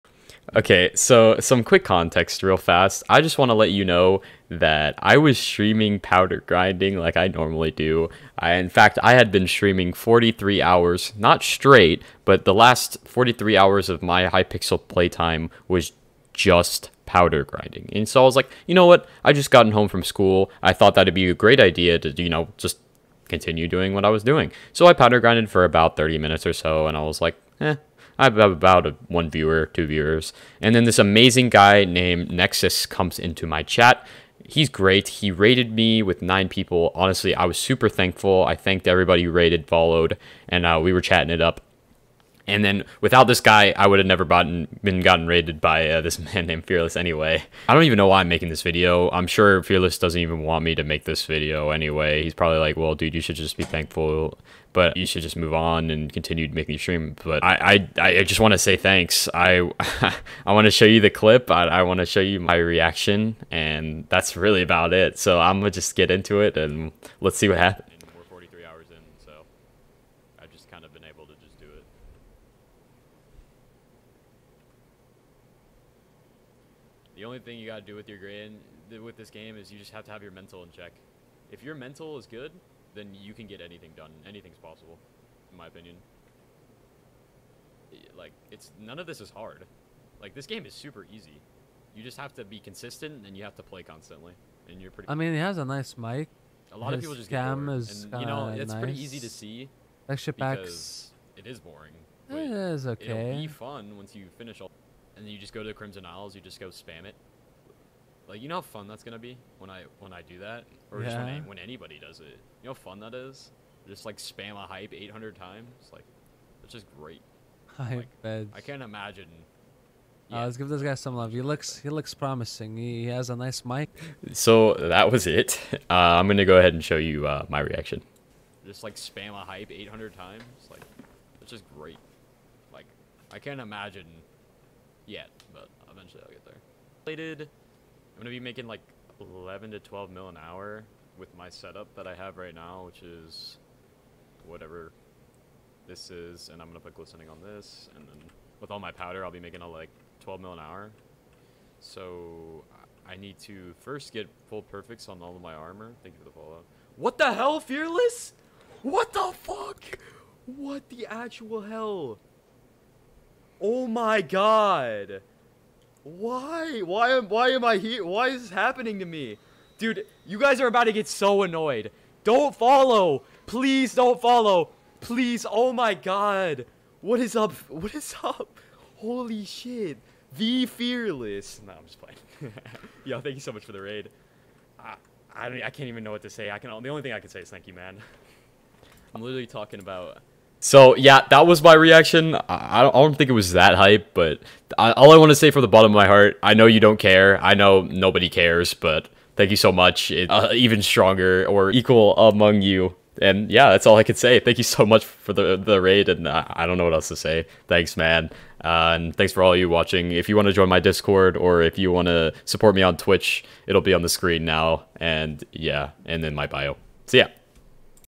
THIS HUGE STREAMER RAIDED ME!
Honestly all I can say is thanks lol! I am not really good at recieving such intense emotions and feelings so this kind of broke me.